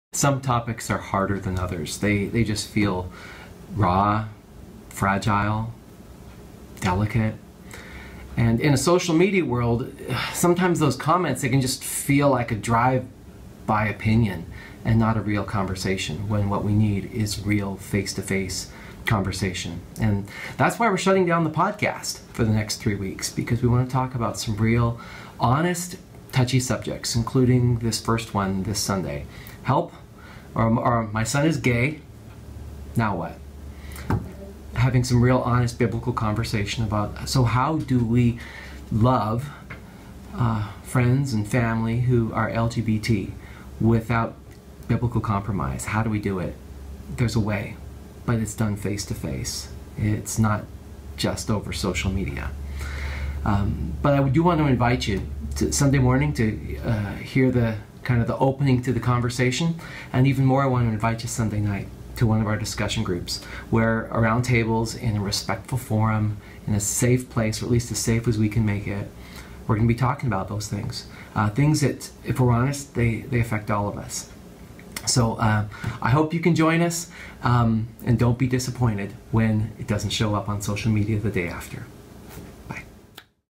Why We're Shutting Down The Podcast (For Three Weeks)
Some topics are harder than others.
They are raw, personal, fragile, difficult.

We live in a Social Media world. But too often social media gives us "drive-by opinions" more than a real conversation. There has to be a way to talk about hard things without our relationships blowing up. There is.

For the next three weeks, we will be taking an honest, biblical look at three especially difficult subjects:

August 19 - "My Son is Gay. What Now?"
How to love LGBT friends and family without biblical compromise.

August 26 - "Help, I'm a Porn Addict!"
Finding help, hope, and healing with our private sins.

September 2 - "Can't We All Just Vote For Jesus?"
How to have political conversations without division.

The morning message will introduce the topic as well as some resources for deeper conversation and biblical study. Then Sunday night, we will offer a Q&A and discussion forum where we can have honest conversations.

To help that happen, we're shutting down our Podcast and YouTube Videocast for three weeks. The recordings and resources will be available upon request if you would like them for your own discussion groups, but here's the thing - we want you to actually talk with someone else about them.

Three weeks of Touchy Subjects starting this Sunday at 9 or 11 am.

Discussion Groups this Sunday night at 6:30 pm.